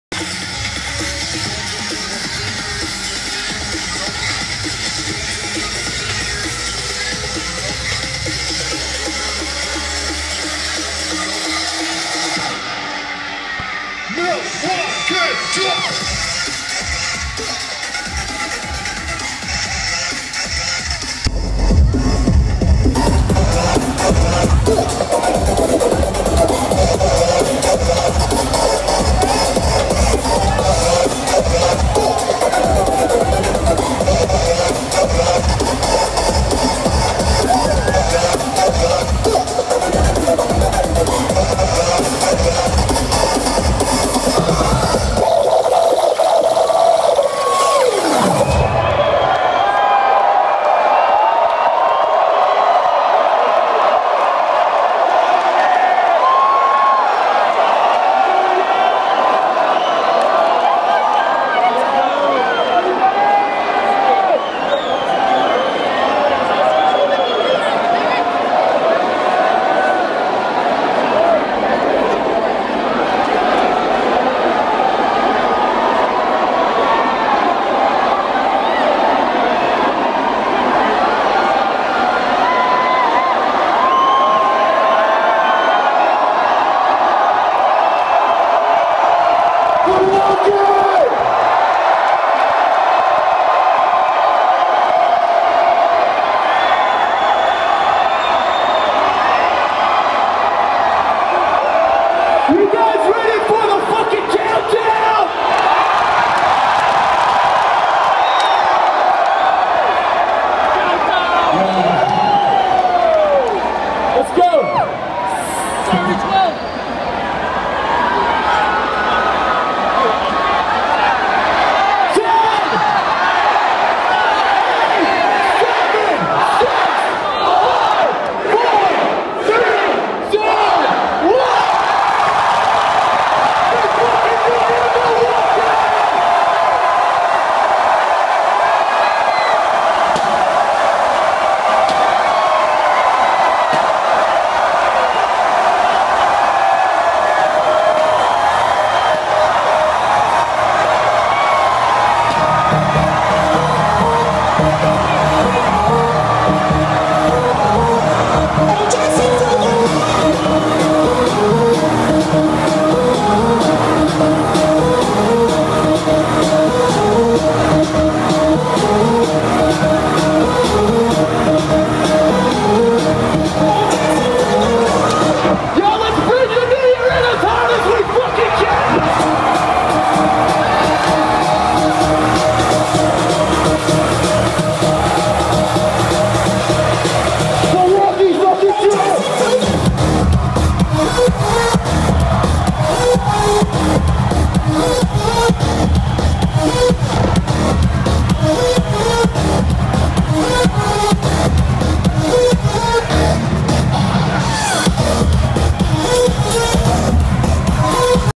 ZOMBOY/ADVENTURE CLUB NYE COUNTDOWN @ THE RAVE 12/31/13
lol balloons failed